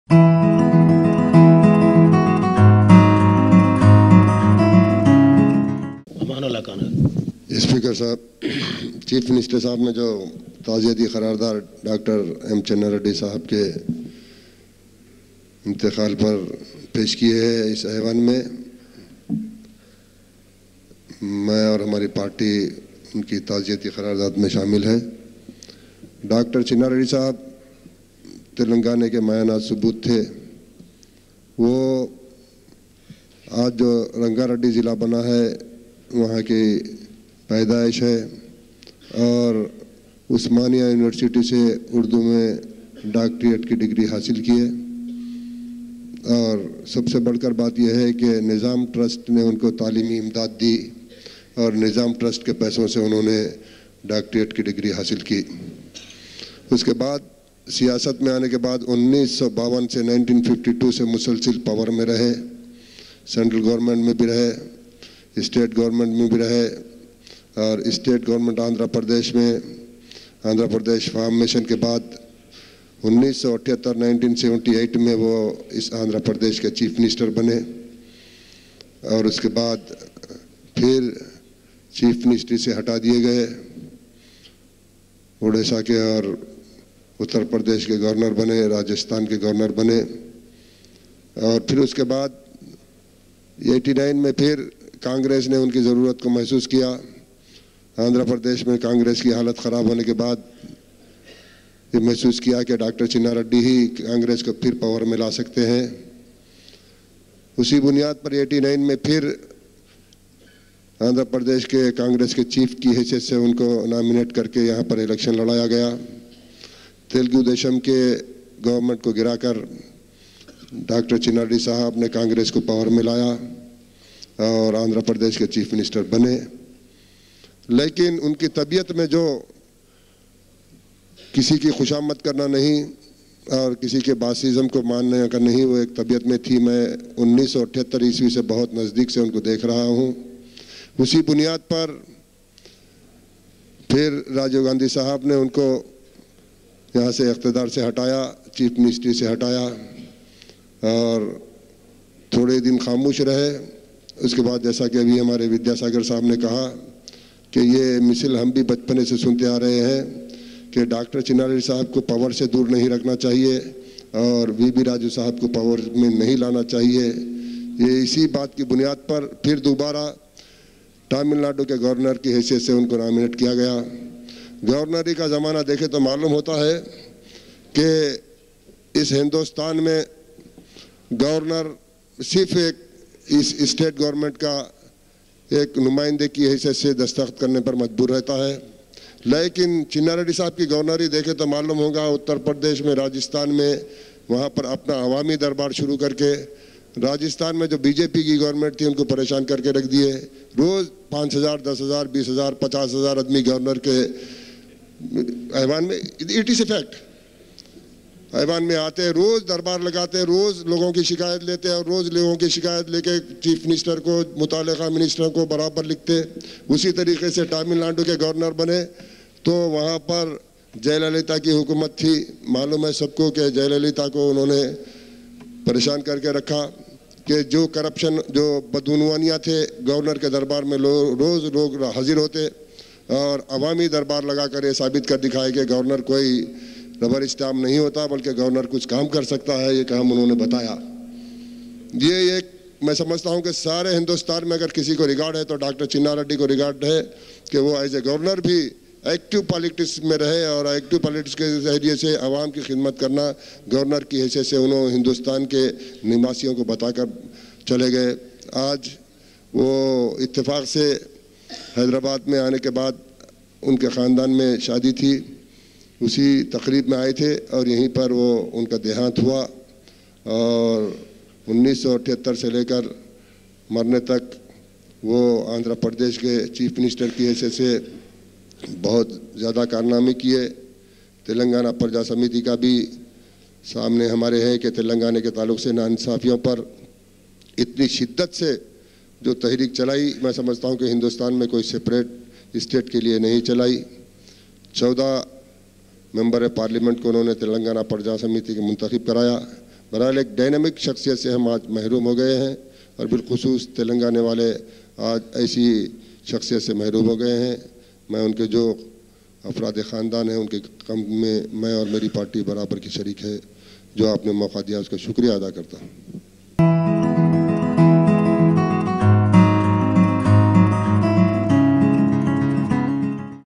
Mohd. Ama Nullah Khan (6) - Dr. Marri Channa Reddy, Condolence Motion In AP Assembly Dec 9, 1996
"Mohd. Ama Nullah Khan, MLA (Mim) (6) speaking during the Dr.
Marri Channa Reddy, Condolence Motion In Andhra Pradesh (AP) Assembly on